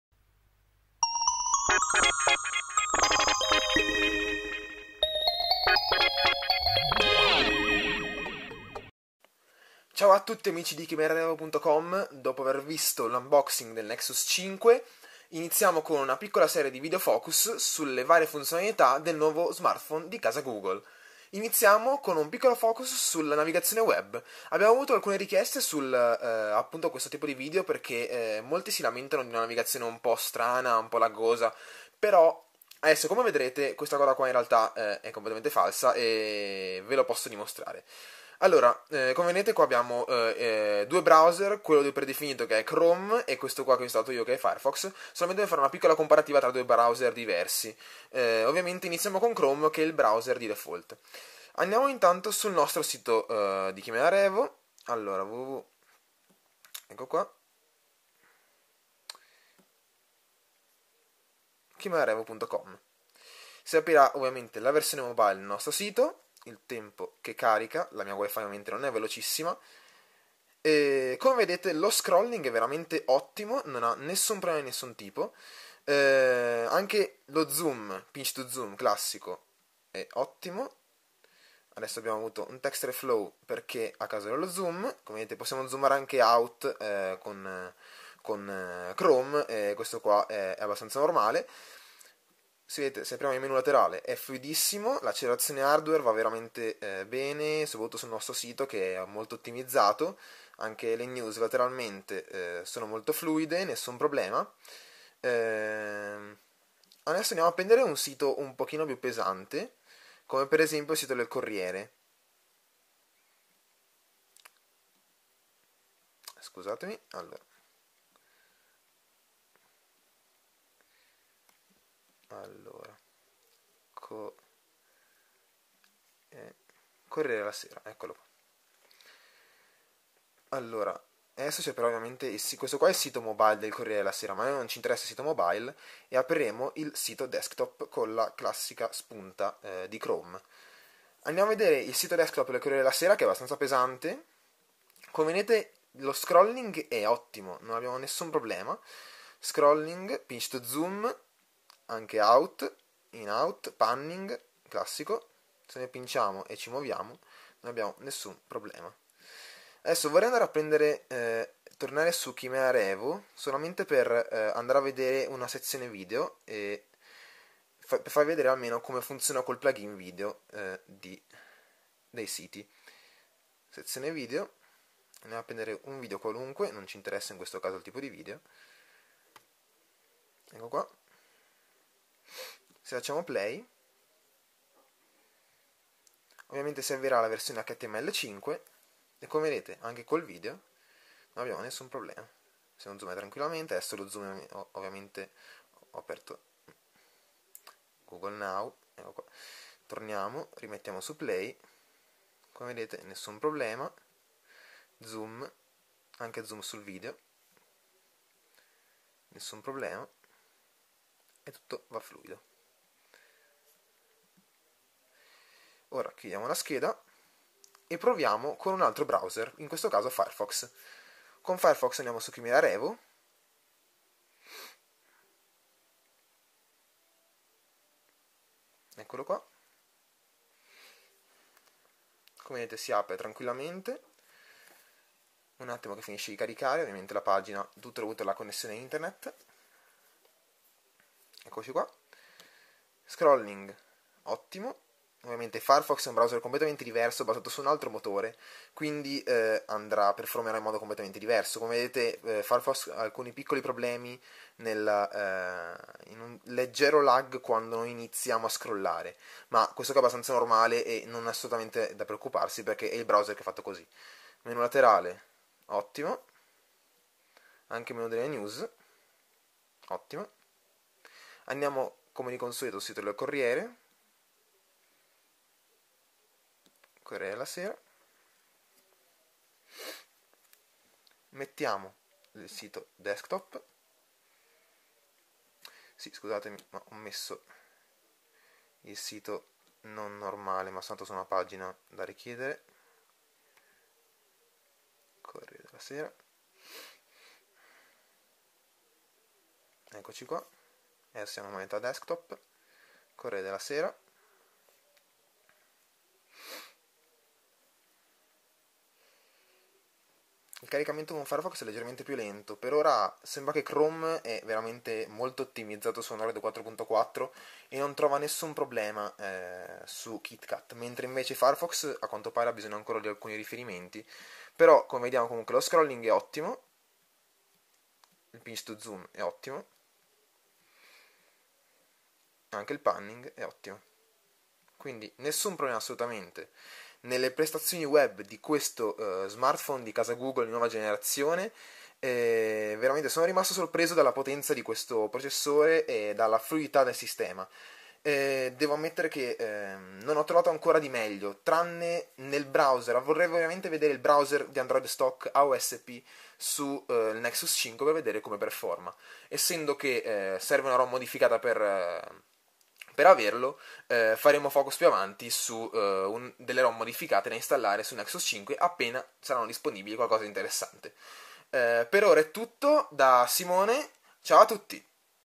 Nexus 5: focus su Web Browsing by ChimeraRevo.com
Il primo della serie di video focus incentrati sul Google Nexus 5 e su Android 4.4 KitKat. In questa puntata sfatiamo il mito della navigazione web laggosa tramite Chrome.